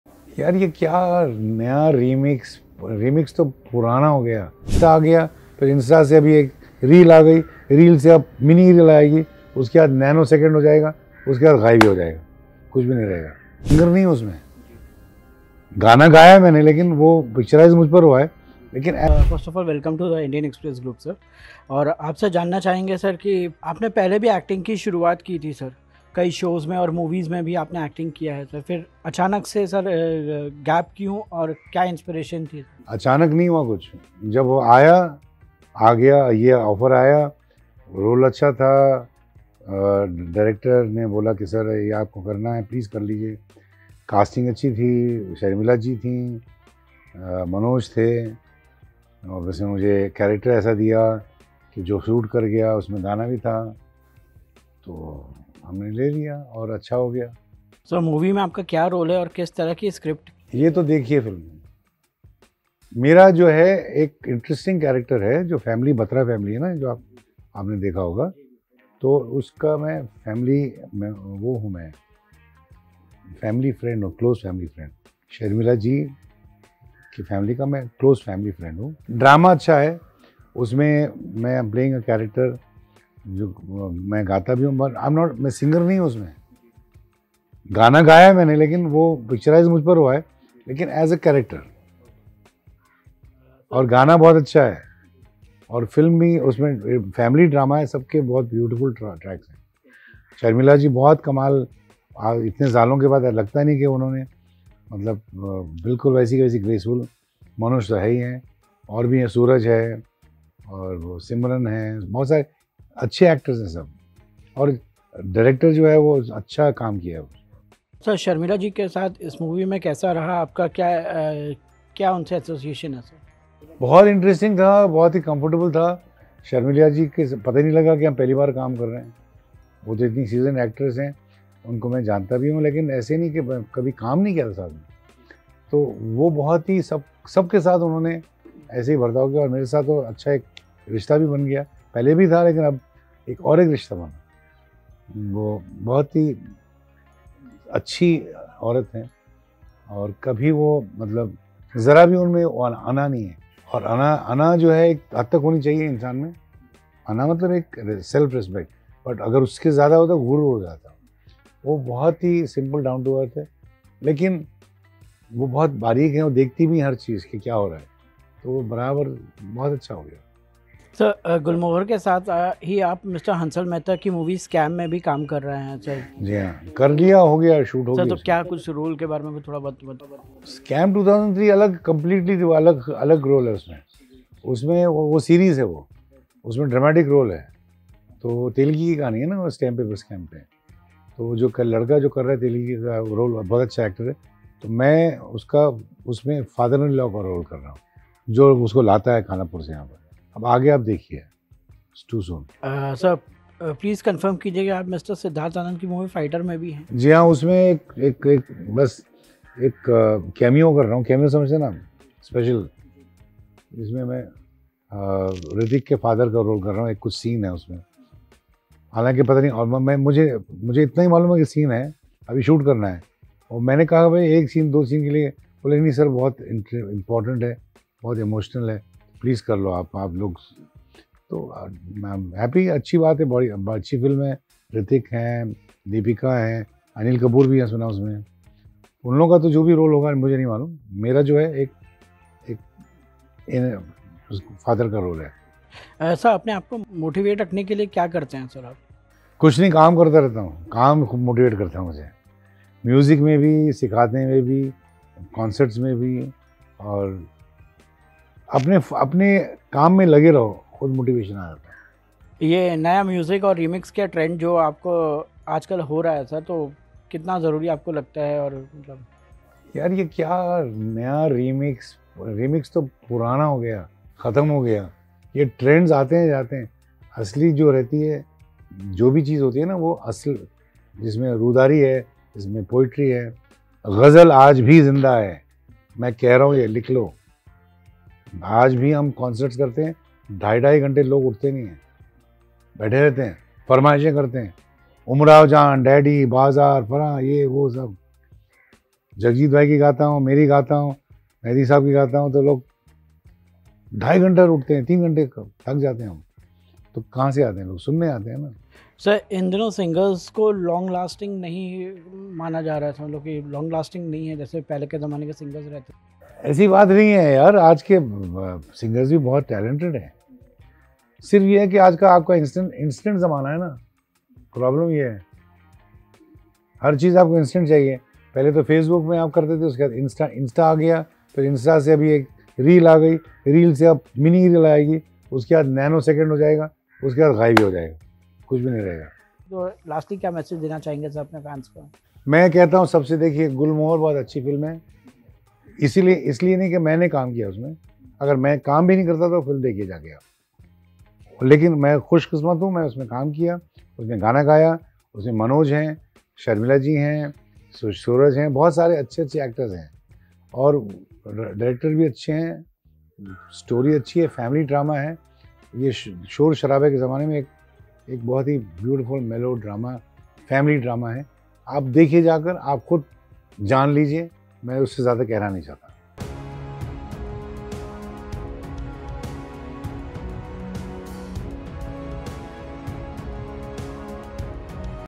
Talat Aziz Interview: Film Gulmohar कितनी खास | Manoj Bajpayee | Talat Aziz Exclusive
Talat Aziz का Exclusive Interview: फिल्म Gulmohar कितनी खास, देखिए पूरा इंटरव्यू(Interview)